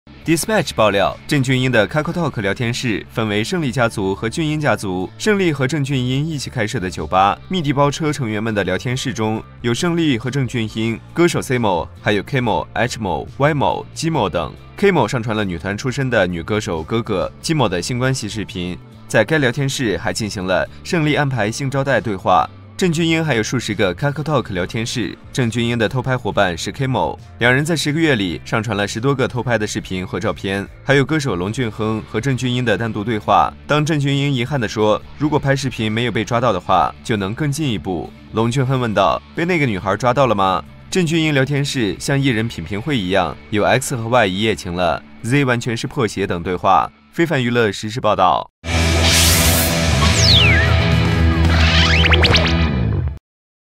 【近期】鄭俊英建數十個聊天室 用于分享偷拍視頻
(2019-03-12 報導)

而截圖中除了有鄭俊英的聊天外，也有歌手勝利的聊天內容，上圖中綠色頭像的李某某便是歌手勝利(原名李勝賢）。而鄭俊英不但發佈視頻炫耀，而且地點也是讓人大跌眼鏡，竟然在葬禮這樣嚴肅的場合上都有，而勝利對於鄭俊英的炫耀也給出積極的回應，稱在能享受到的時候盡情去享受。